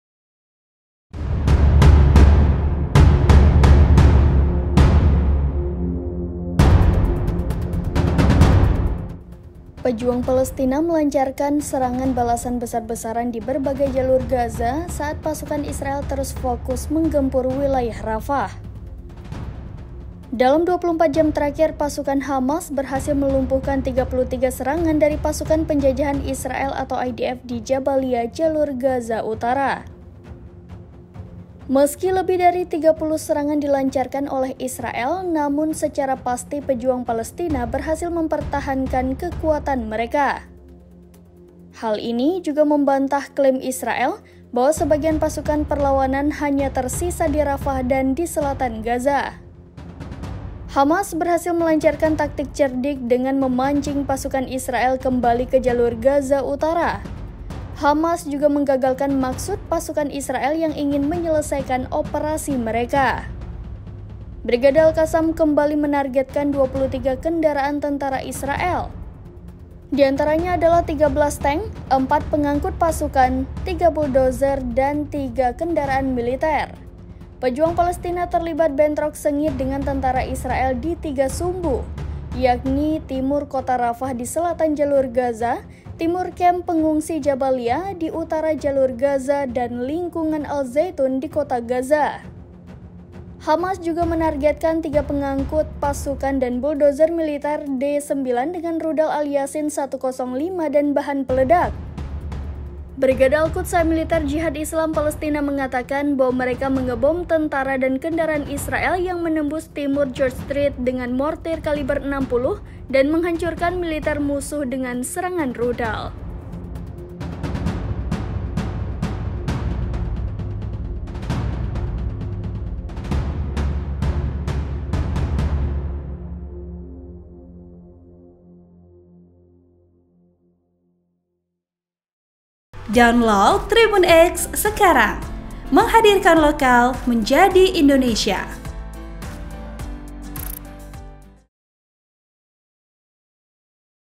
Tiga Poros Pendudukan Israel Digempur Pasukan Hamas, Puluhan Tak IDF Dihancurkan
SERAMBINEWS.COM - Pejuang Palestina melancarkan serangan balasan besar-besaran di berbagai jalur Gaza saat pasukan Israel terus fokus menggempur wilayah Rafah. Dalam 24 jam terakhir, pasukan Hamas berhasil melumpuhkan 33 serangan dari pasukan penjajahan Israel (IDF) di Jabalia, jalur Gaza Utara.

Meski lebih dari 30 serangan dilancarkan oleh Israel, namun secara pasti, pejuang Palestina berhasil mempertahankan kekuatan mereka. Hal ini juga membantah klaim Israel bahwa sebagian pasukan perlawanan hanya tersisa di Rafah di selatan Gaza.

Hamas berhasil melancarkan taktik cerdik dengan memancing pasukan Israel kembali ke jalur Gaza Utara. Hamas juga menggagalkan maksud pasukan Israel yang ingin  menyelesaikan operasi mereka. Brigade Al-Qassam berhasil menargetkan 23 kendaraan tentara Israel.

Di antaranya adalah 13 tank, 4 pengangkut pasukan, 3 buldoser dan 3 kendaraan militer. Pejuang Palestina terlibat bentrok sengit dengan tentara Israel di tiga sumbu. Yakni timur kota Rafah di selatan Jalur Gaza, timur kamp pengungsi Jabalia di utara Jalur Gaza, dan lingkungan al-Zaytoun di Kota Gaza.

Hamas juga menargetkan 3 pengangkut pasukan, dan Buldoser militer D-9 dengan rudal Al-Yassin 105 dan bahan peledak.

Brigade al-Quds, sayap militer Jihad Islam Palestina, mengatakan bahwa mereka mengebom tentara dan kendaraan Israel yang menembus timur George Street dengan mortir kaliber 60, dan menghancurkan militer musuh dengan serangan rudal. (*)